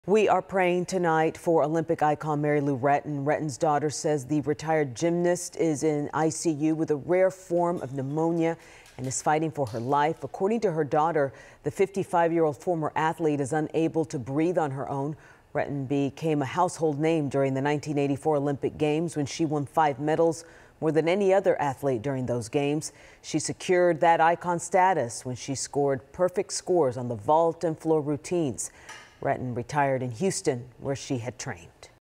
Olympic gymnast Mary Lou Retton 'fighting for her life' in ICU, daughter says
McKenna Kelley says her mom, who lives in Houston, has a very rare form of pneumonia and is not able to breathe on her own.